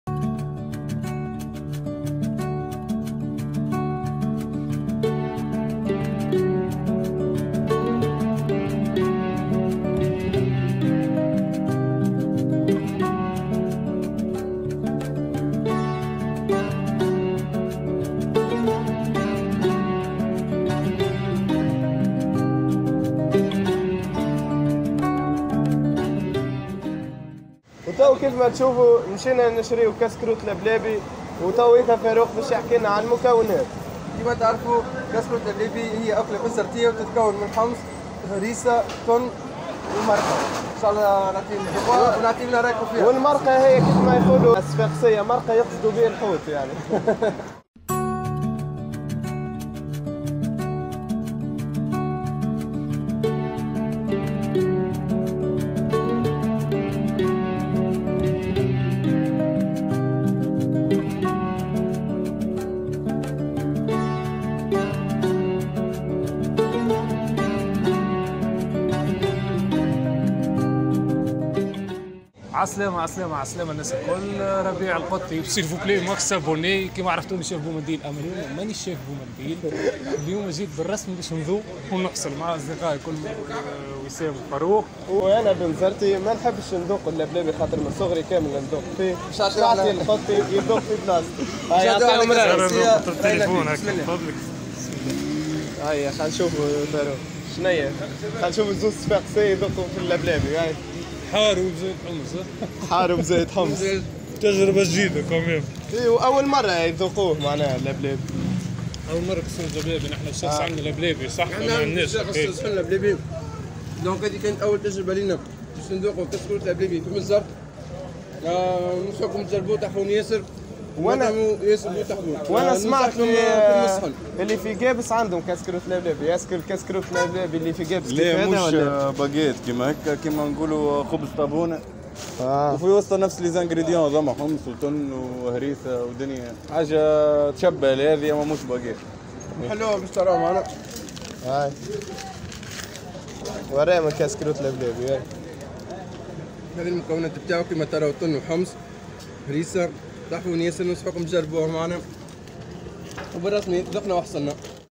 كسكروت لبلابي بنزرتي بنة عالمية 👌
!!! جولة اكل الشوارع في مدينة بنزرت 🔥 الأكل البحري و الكسكروت لبلابي

 بنزرت مدينة جميلة موقعها أقصى شمال افريقيا و أكلها البحري المتنوع مع الكسكروت اللبلابي الفريد من نوعه😘

dhouk tohsel

Wissaa

Farouk